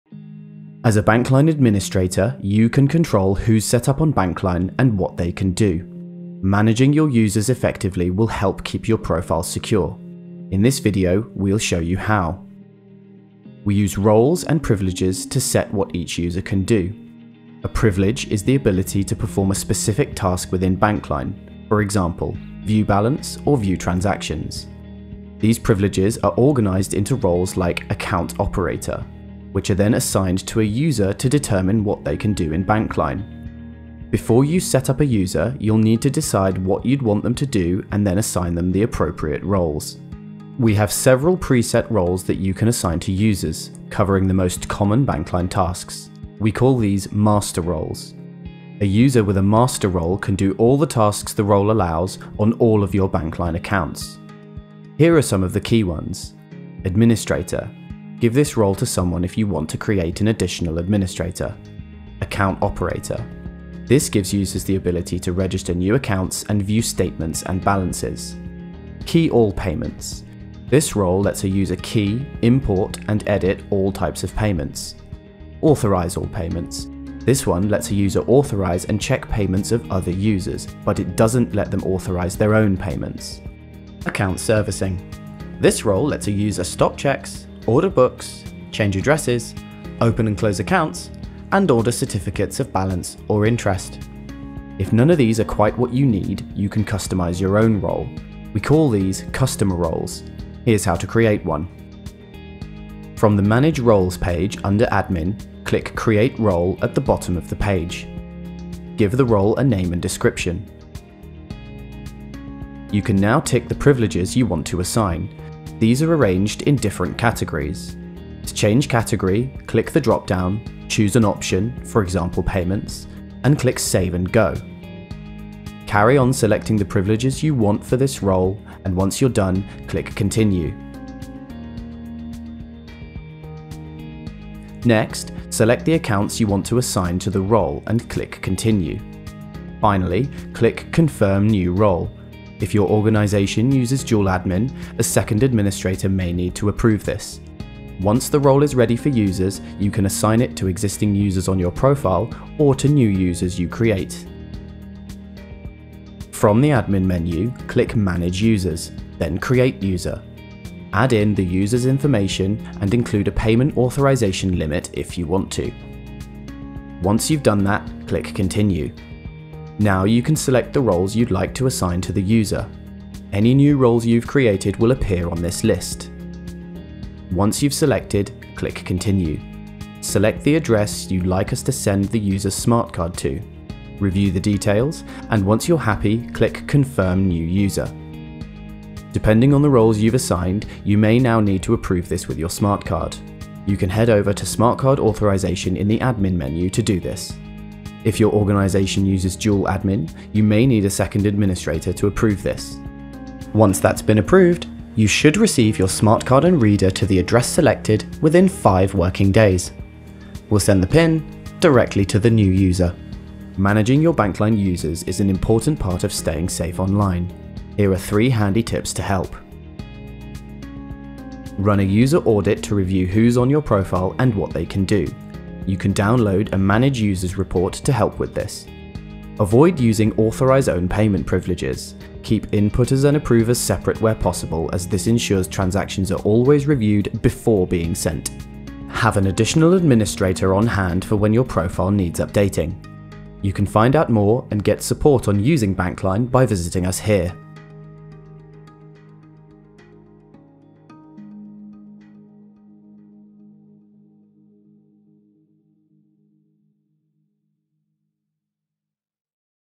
Bankline Admin - Roles and Users | NatWest
As a Bankline administrator, you can learn about how to control who is set up on Bankline and what they can do. Managing your users effectively will help keep your profile secure.

where you can find more videos, blog articles, guides, and free webinars to help you get the most out of your Bankline profile

-timestamps-

00:00 - Intro & overview

00:13 – Roles and privileges

00:41 – Managing roles & Master Roles

01:38 – Customer roles

02:34 – Creating a user

03:33 – Tips for managing users